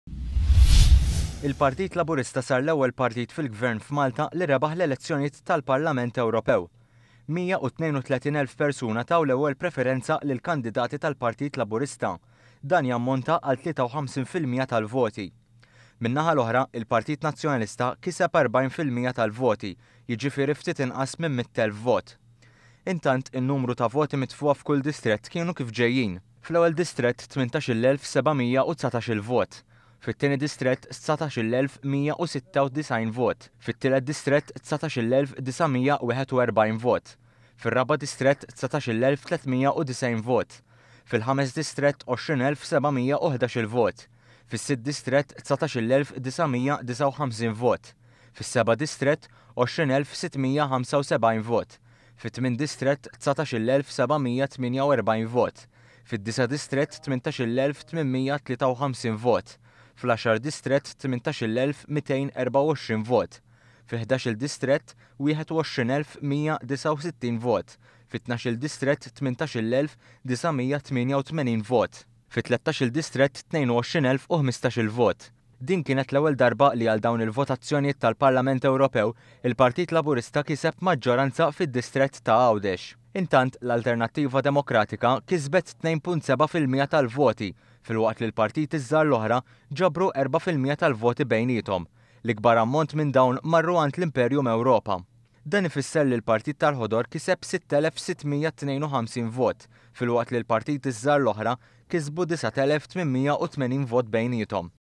Il-Partit Laburista jikseb 53% tal-voti fl-elezzjoni tal-Parlament Ewropew
Il-Partit Laburista kiseb tlieta u ħamsin fil-mija tal-voti fl-elezzjoni tal-Parlament Ewropew.Dan fisser rebħa storika għall-Partit Laburista li huwa l-ewwel partit fil-gvern li rebaħ dawn l-elezzjonijiet